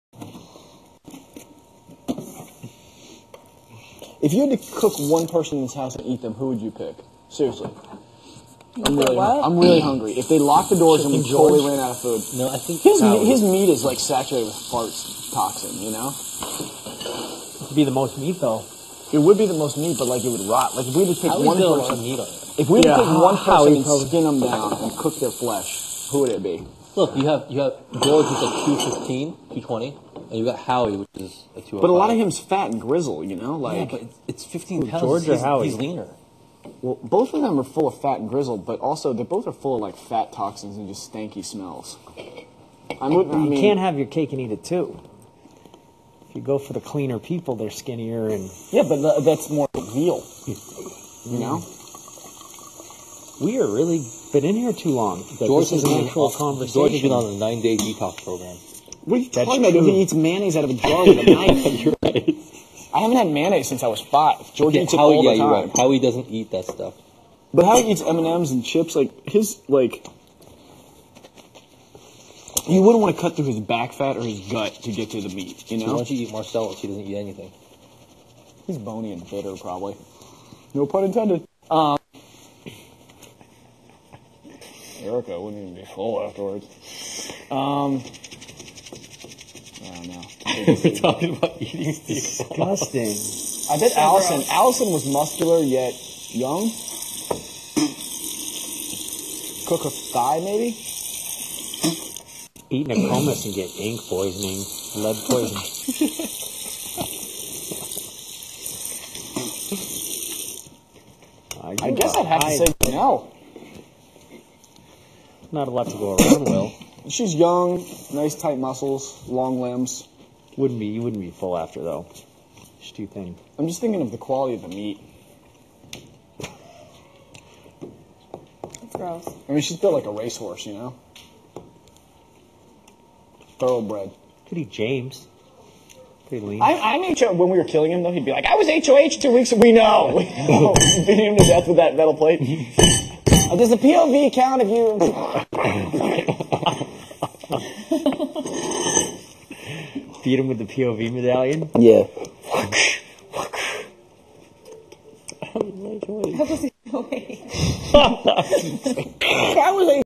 Which HG Would You Eat?
Week 4, Will, being on slop and hungry, asks which HG would they eat. Will says he would eat Janelle and Boogie says James.